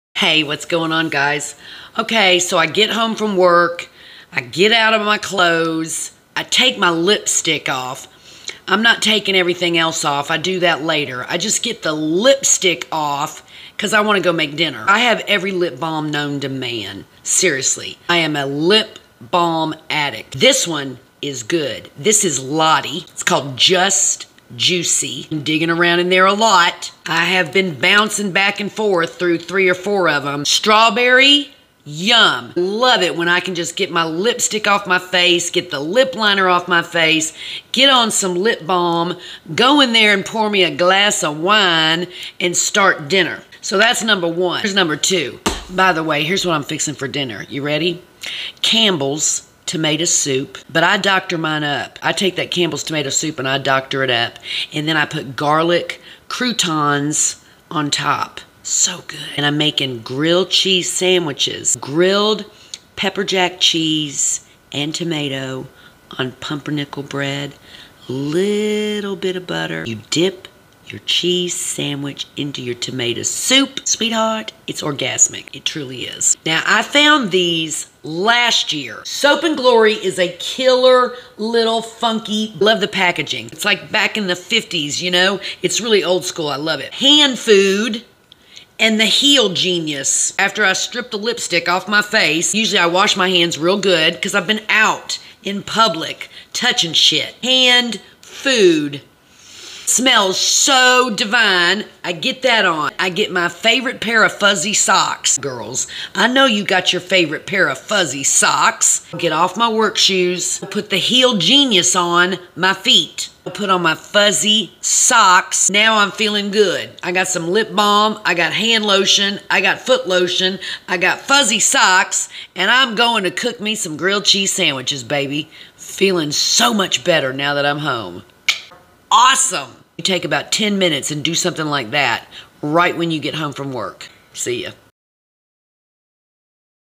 SOAP & GLORY! GLASS OF WINE! FUZZY SOCKS! GRILLED CHEESE! TOMATO SOUP! WIND DOWN AFTER HARD DAY!
SOAP & GLORY HAND FOOD & HEEL GENIUS ROCKS!